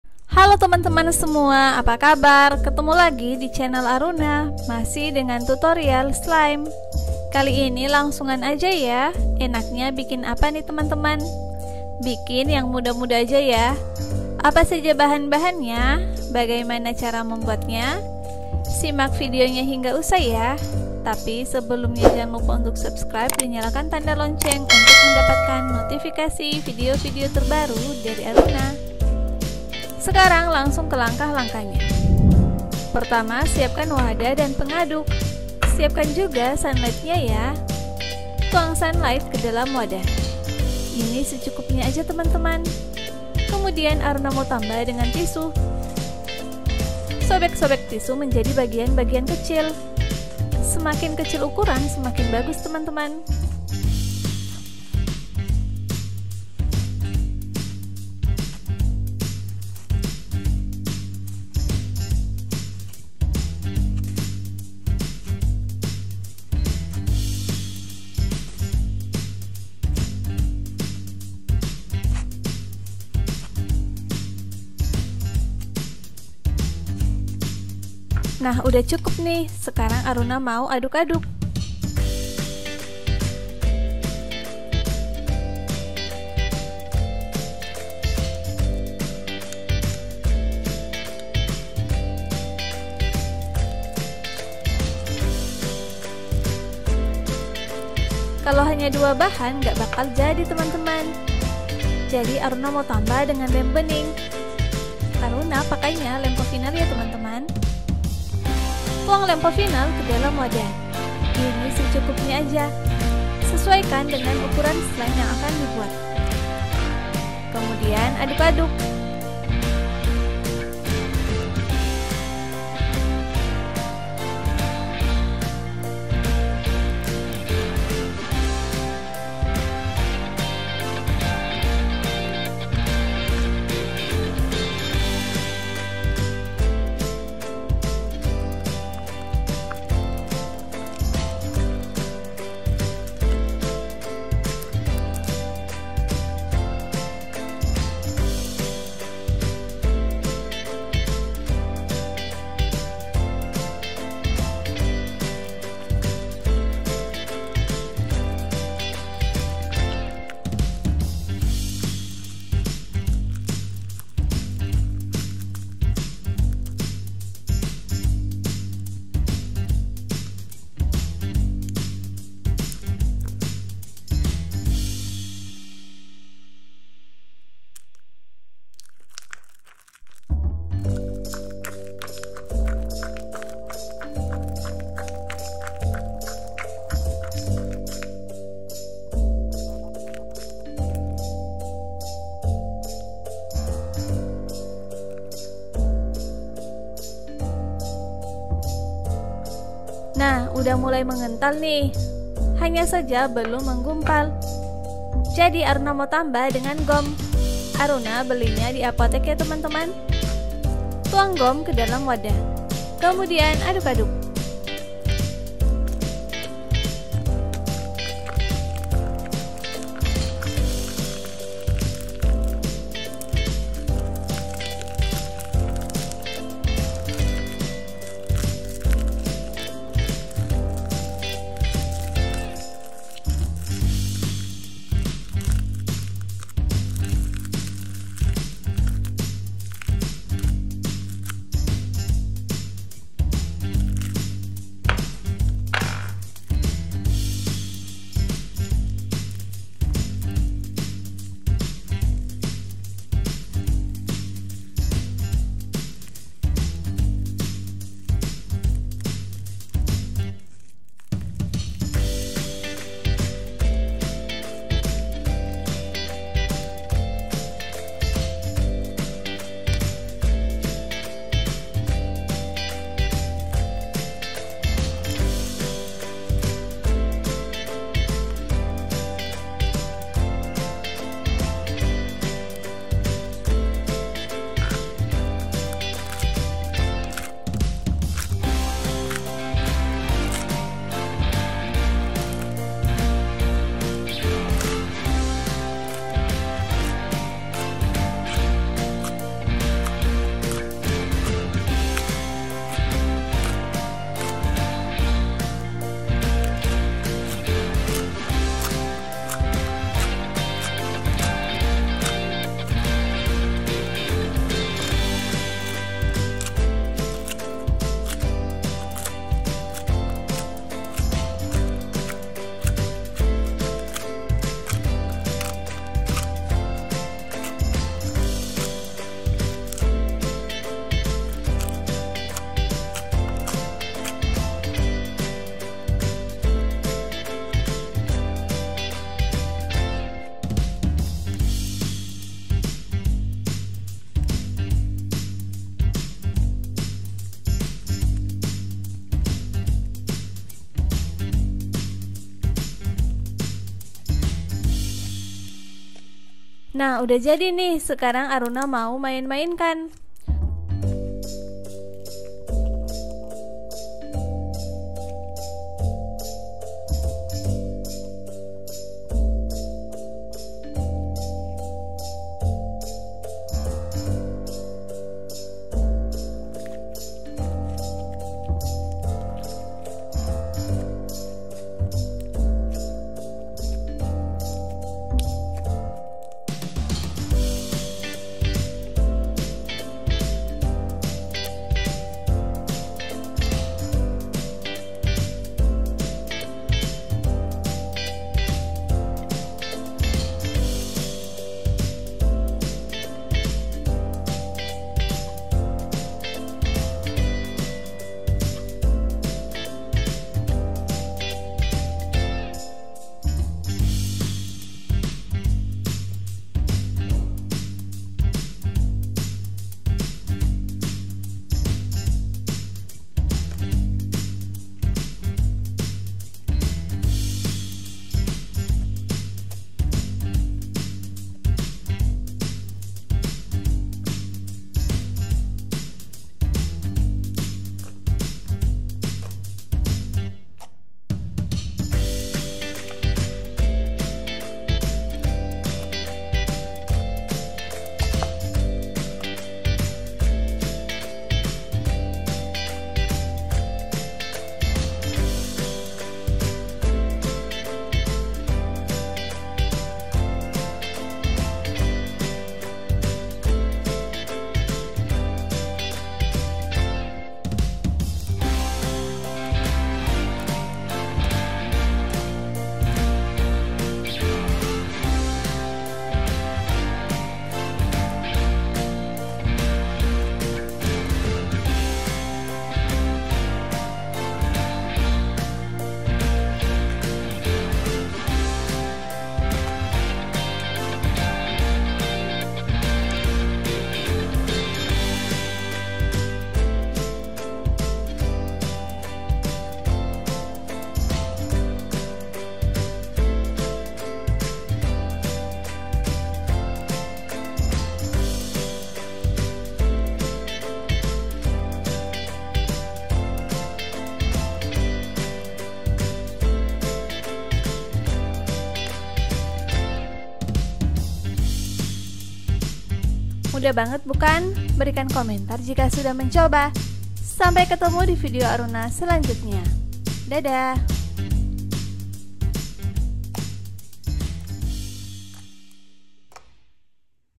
Cara Membuat Slime 2 Bahan Dari Tisu Dan Sunlight
Hallo teman-teman semua, kali ini Aruna mau share Cara Membuat Slime 2 Bahan Dari Tisu Dan Sunlight

Bagaimana cara membuatnya??? Simak terus videonya hingga usai yah.

Simak juga video video Aruna lain tentang, cara membuat slime, cara bikin slime, dan cara membuat slime lainnya.